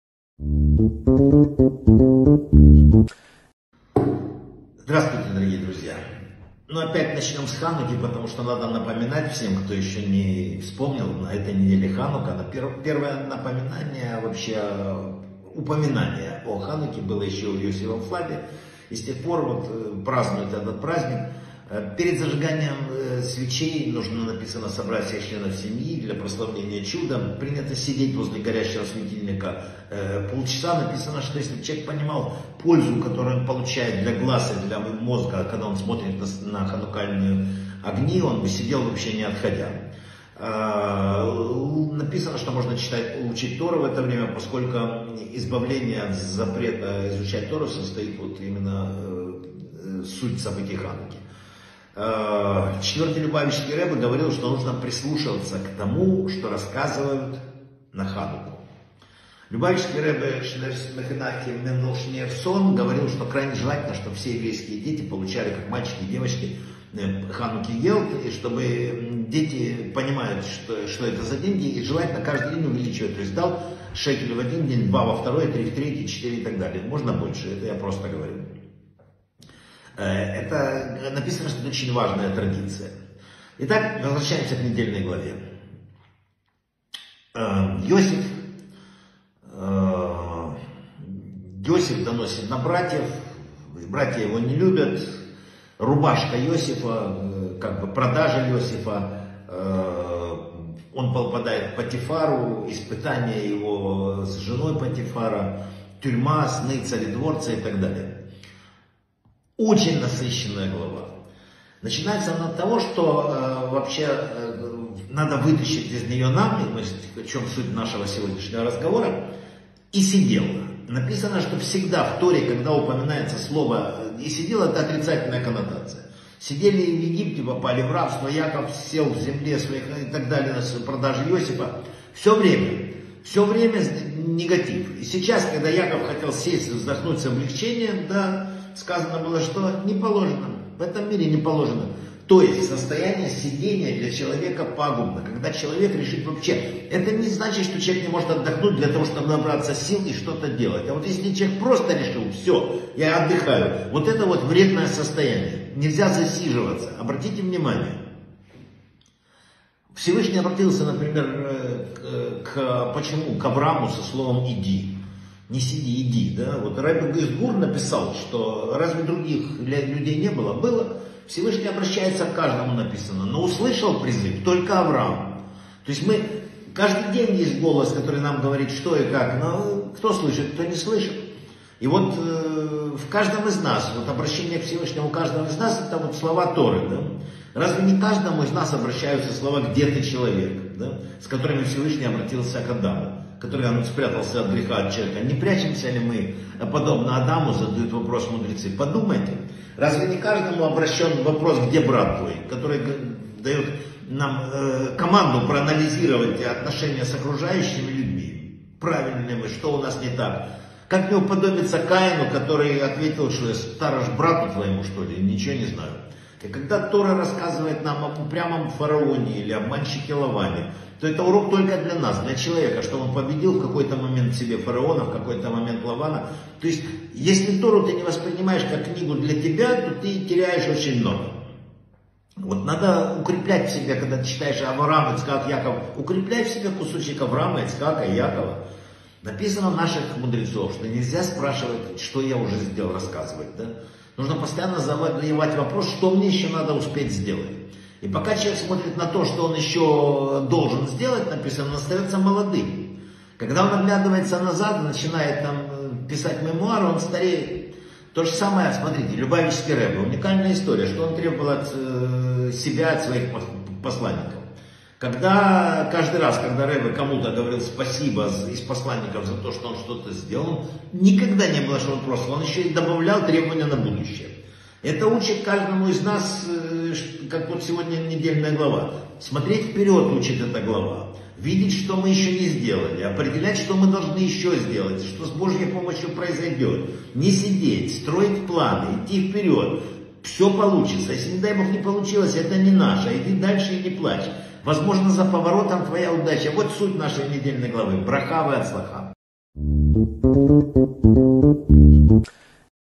📘 Почему человеку нельзя засиживаться на месте? Недельная глава Ваешев - Урок 1 | Вадим Рабинович
Еще немного о Хануке - какую духовную пользу несет человеку свет праздничных свечей? Чем лучше всего заниматься в дни праздника?
Что говорили об обычаях Хануки четвертый и седьмой Любавичские Ребе?
Сложные отношения Йосефа с братьями и его продажа в Египет стали сильным потрясением для его отца Яакова. На что нам намекает самое начало главы?
Почему состояние сидения на месте для нас является крайне вредным и пагубным?
Голос Вс-вышнего взывает к нам каждый день - не прячемся ли мы от Него?
Каждый эпизод Торы - урок для человека о борьбе с внутренним Каином, Лаваном, фараоном и так далее.
Важна концентрация не на том, что уже сделано, а на том, что еще можно успеть сделать. Что требовал Ребе от себя и от своих посланников?